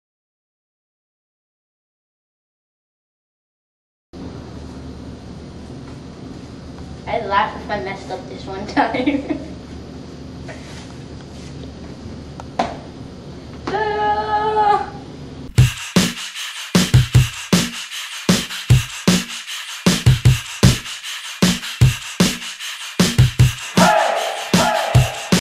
Through the head of a racquet!
Watch this young man put his body through the head of a Karakal SX 100 racquet!  That is 470 squash cm